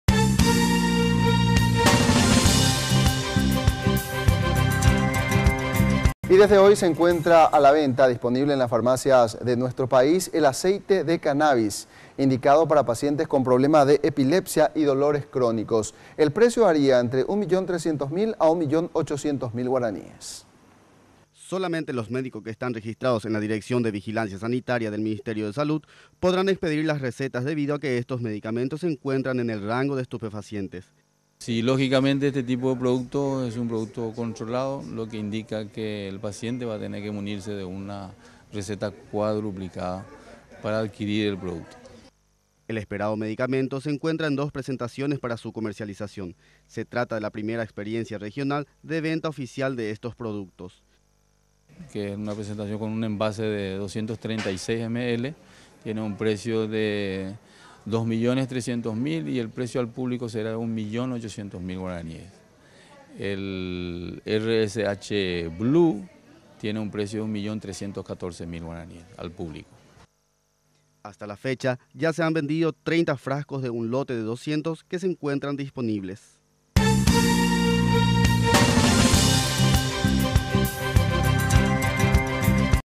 Aceite de cannabis ya está a la venta en el país
Desde hoy se encuentra a la venta en farmacias de nuestro país, el aceite de cannabis importado directamente de Estados Unidos para pacientes que sufren de epilepsia y dolores crónicos. El precio varía entre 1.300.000 y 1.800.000 guaraníes cada frasco.